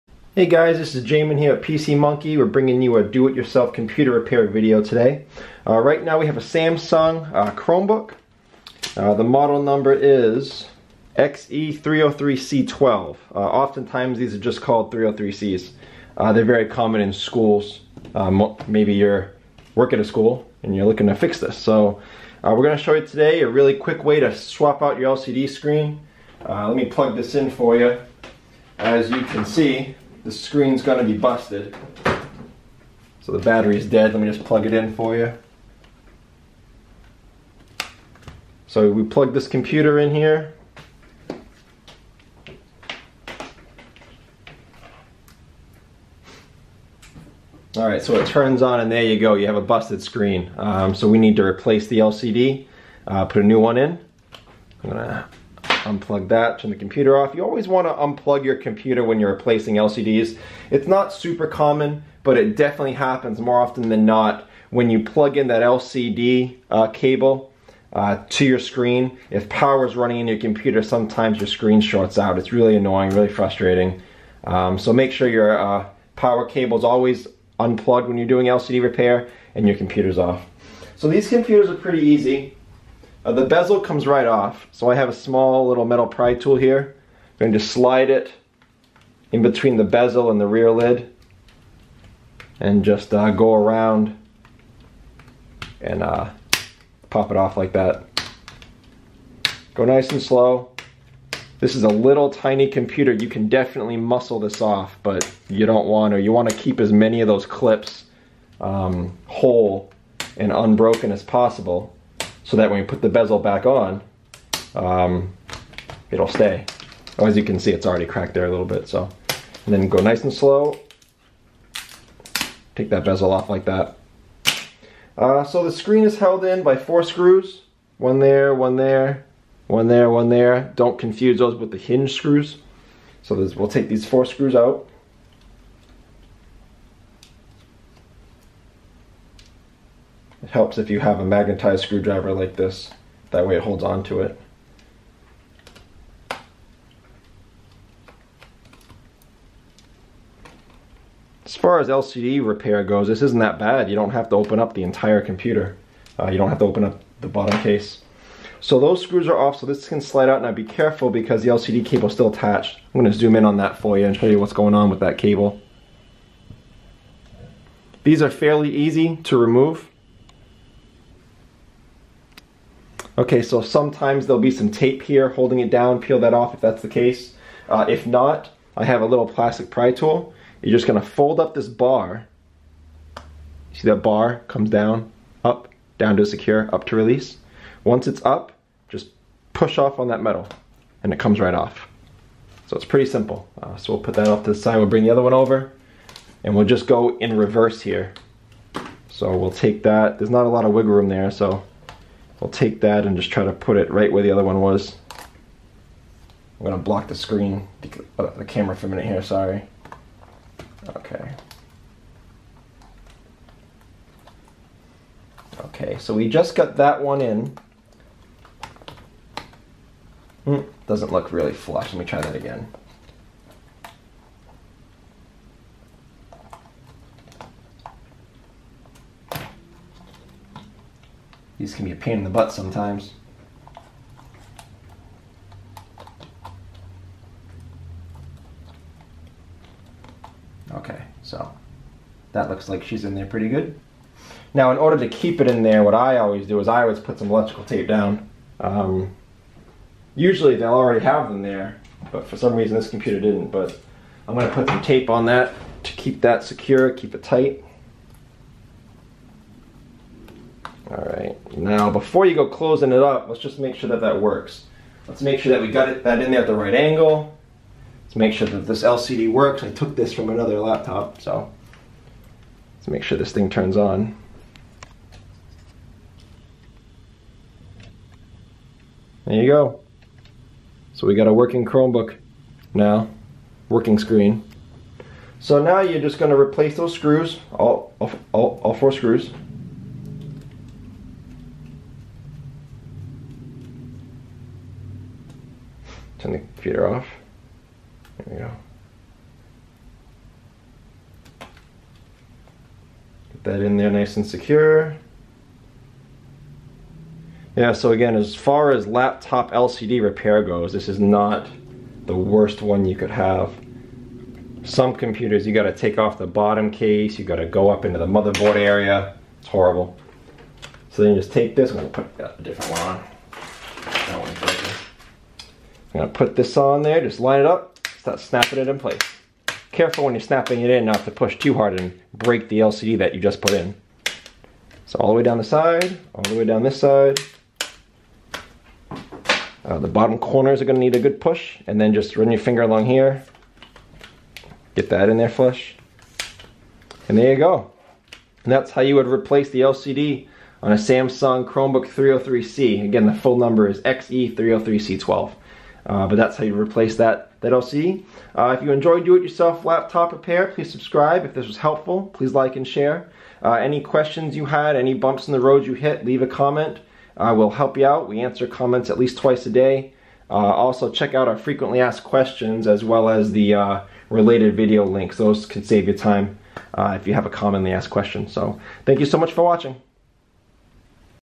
How To Fix Replace LCD Screen - Samsung Chromebook 303C
Replacing the screen in Samsung Chromebook XE303C12 can be easier than you think.  Use this video to show you how.

FAQ's
Q:  What size screen is that?
A:  11.6 inch